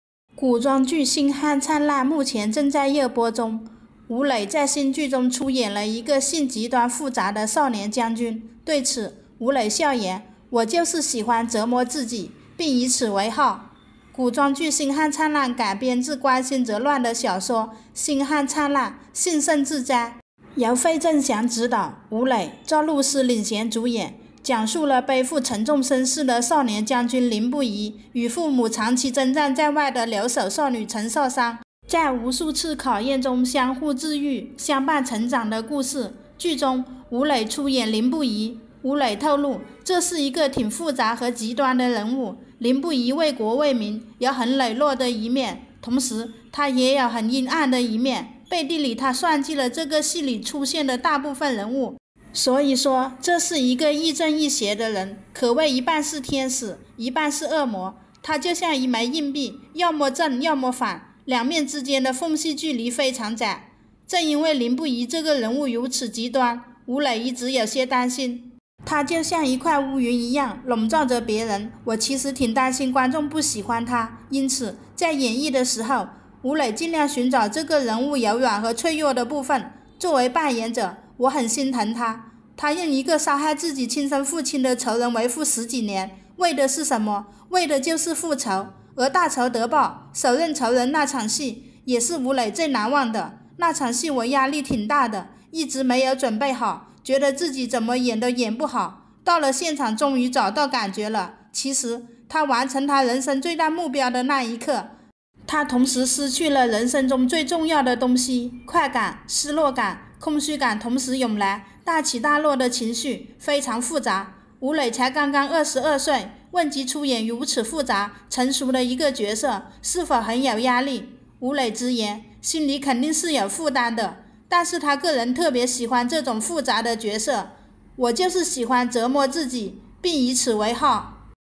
《星汉灿烂》吴磊：挺担心观众不喜欢凌不疑
Wu Lei: I'm Worried That the Audience Doesn't Like Ling Buyi